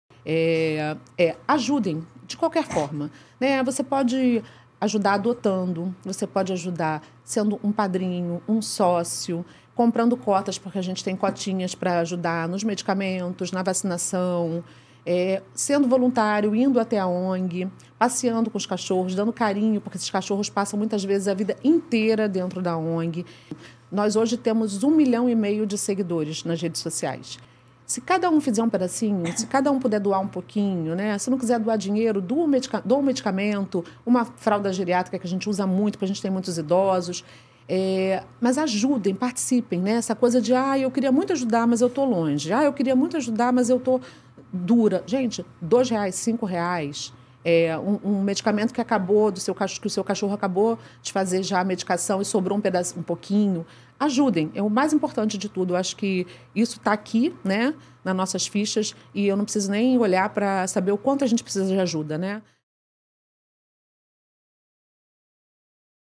CORTE PODCAST EM MOVIMENTO
Estão preparados para esse Podcast falando sobre os 13 anos de ONG Paraíso dos Focinhos?! Então corre lá no nosso canal do Youtube que o episódio está completinho   E Para vocês já sentirem o gostinho do que foi esse bate-papo, deixamos um pouquinho das histórias do Paraíso aqui.